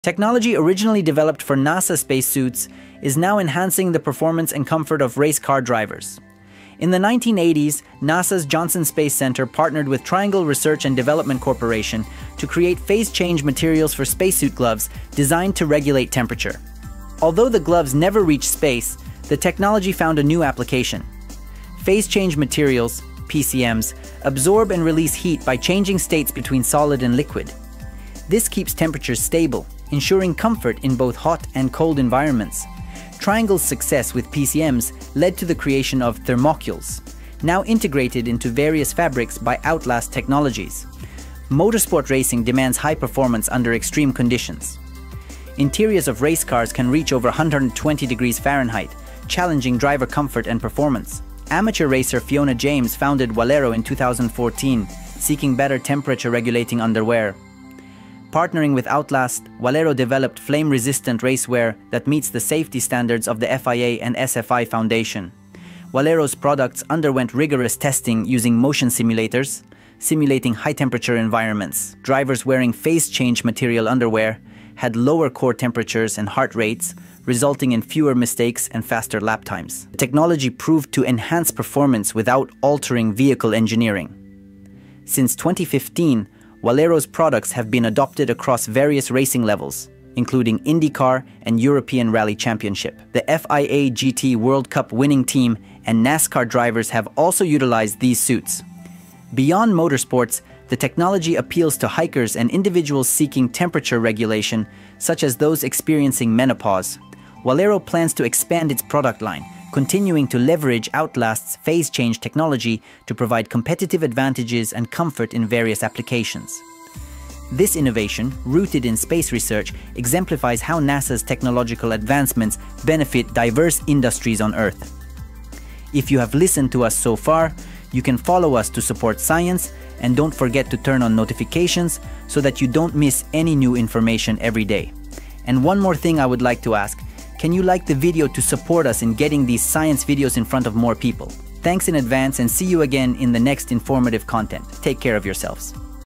The Next Generation Spacesuit | This technology is incredible!
Despriction:

In the 1980s, NASA developed phase change materials (PCM) for astronaut gloves to regulate temperature. This technology has now enhanced race car drivers' performance and comfort. Outlast Technologies integrated PCMs into thermal fabrics. In 2014, amateur racer Fiona James founded Wero to develop better temperature-regulating underwear. Partnering with Outlast, they designed flame-resistant racewear meeting FIA and SFI safety standards. This technology reduced drivers' body temperatures and heart rates, enhancing performance. Since 2015, Wero's products have been used across various racing levels. This innovation demonstrates how NASA's technological advancements benefit diverse industries on Earth.